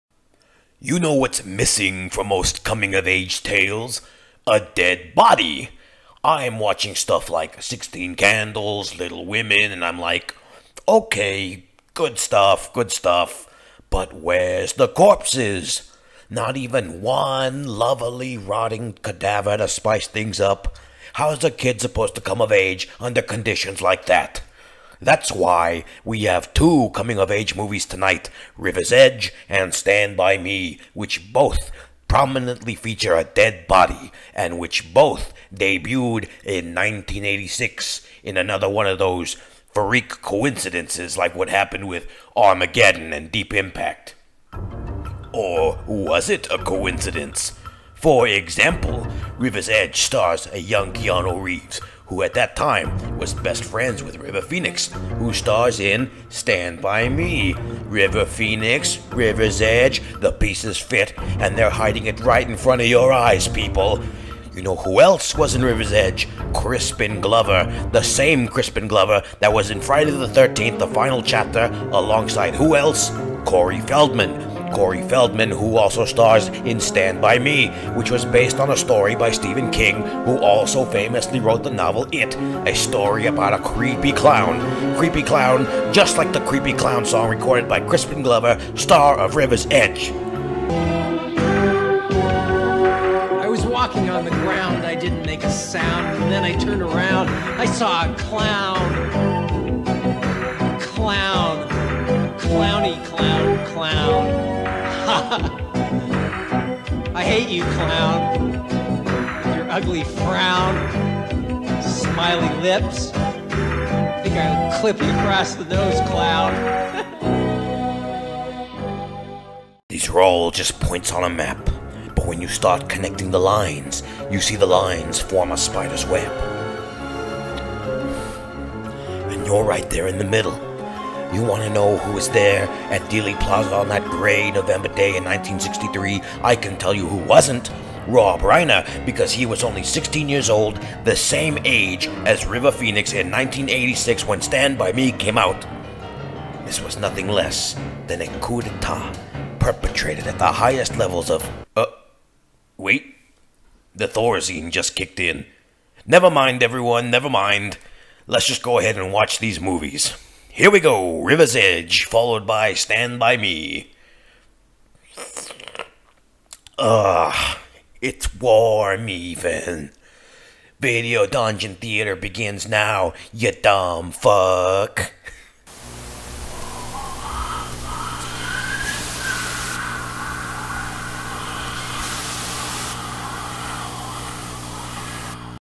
VDT Live Show Intro: "River's Edge/Stand By Me" (July 2021)
Tapehead introduces the movies for Video Dungeon Theatre's July 2021 live show at the Oak Hills Tavern in San Antonio, TX.